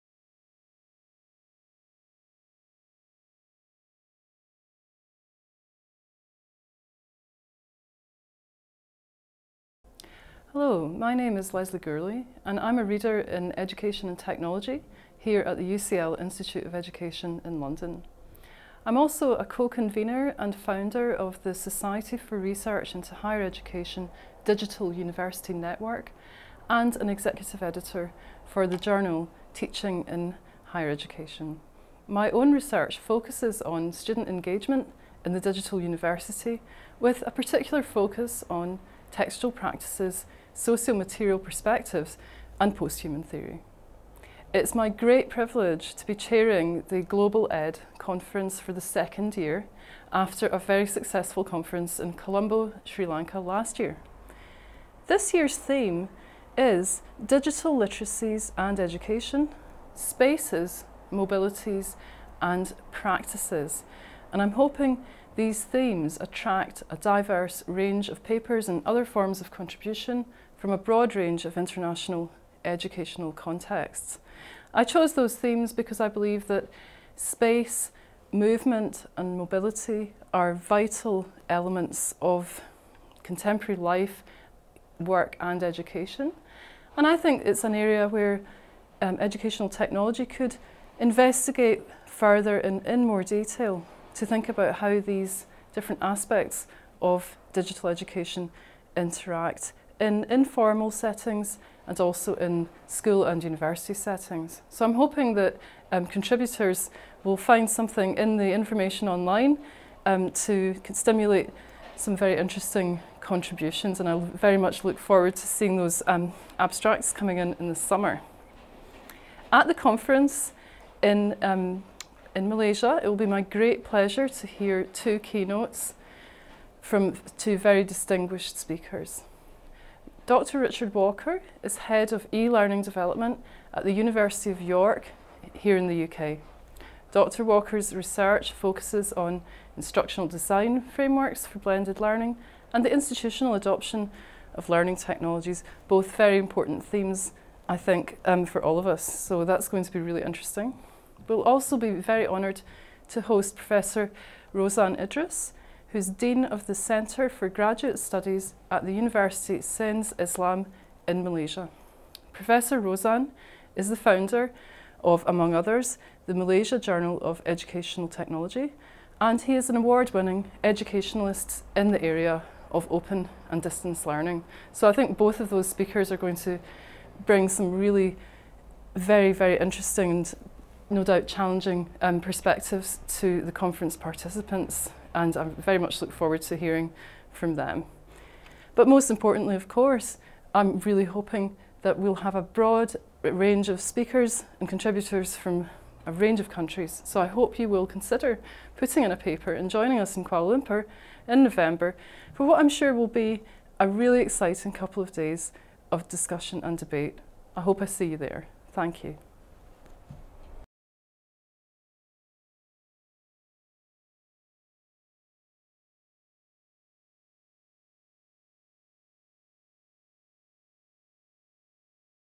Global Education E – Learning Conference 2018 | Co-chairs Dr. Lesley Gourlay
Call for Paper is open now!!

Welcome to the 2nd International Conference on Global Education and E -Learning (GLOBED 2018) to be held from 15th and 16th November 2018 in Kuala Lumpur, Malaysia under the theme “Digital Literacies and Education: Spaces, Mobilities and Practices”.